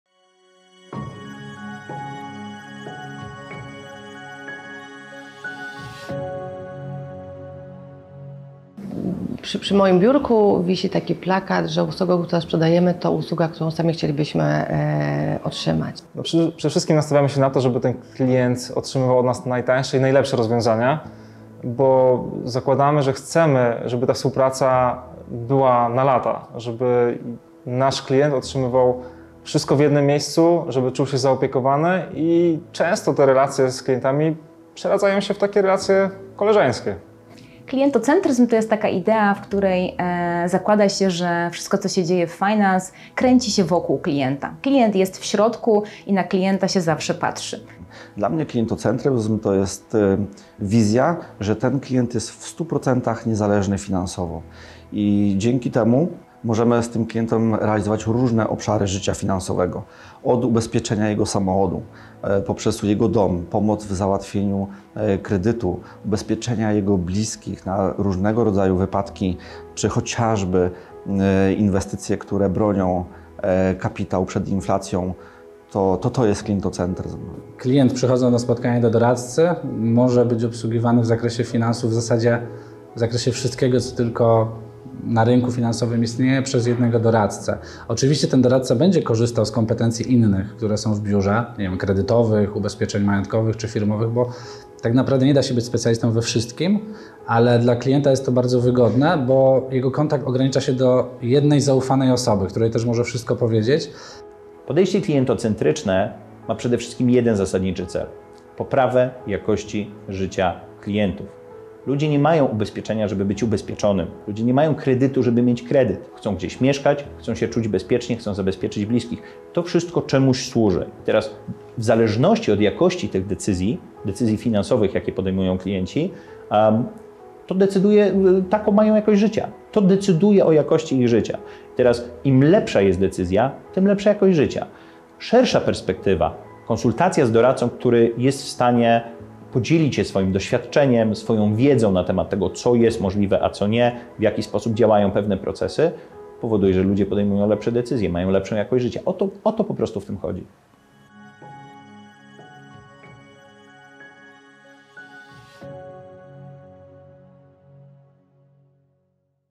Klientocentryzm
O stawianiu klienta w centrum swoich działań mówi otwarcie prawie każda firma usługowa. Często jednak jest to niczym niepoparta idea, która dobrze brzmi. Dla nas klientocentryzm jest wielowymiarowy. Pokazuje klienta w centrum ale też otwiera wiele możliwość naszym w współpracownikom. Wiecie skąd wzięła się idea klientocentryzm? Nie? Zatem zapraszam do obejrzenia materiału.